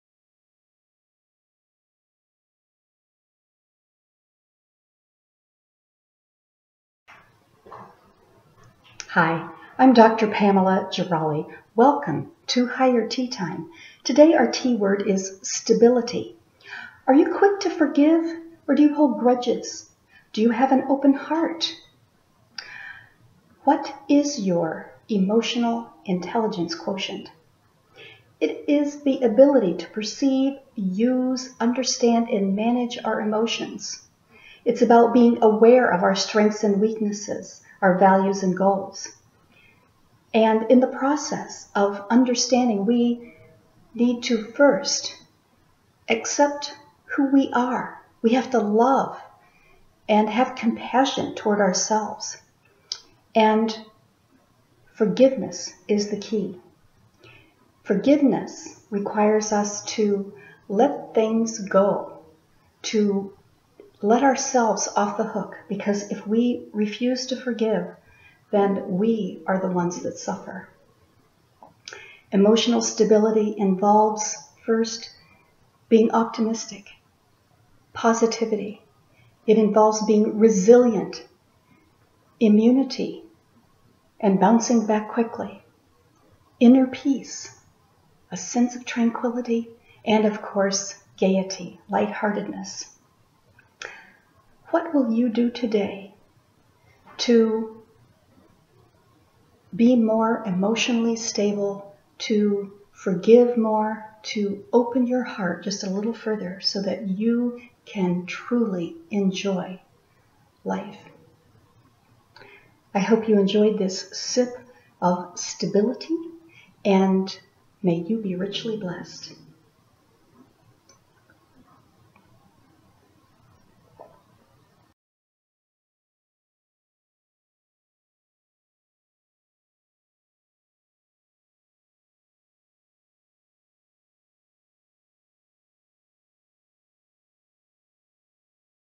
Emotional StabiliTea: Heal & Open the Heart! 2:18
Dr. Pamela Gerali shares insights about how to become more stable and secure from the Blueprint for the Human Spirit.